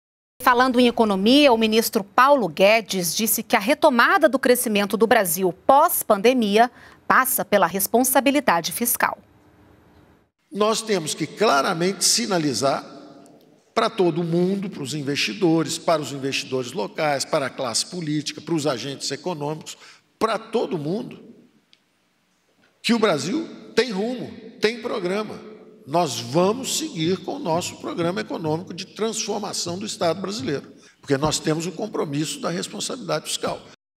Guedes defende responsabilidade fiscal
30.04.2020 - O ministro da Economia, Paulo Guedes, disse que a retomada do crescimento do Brasil pós-pandemia passa pela responsabilidade fiscal.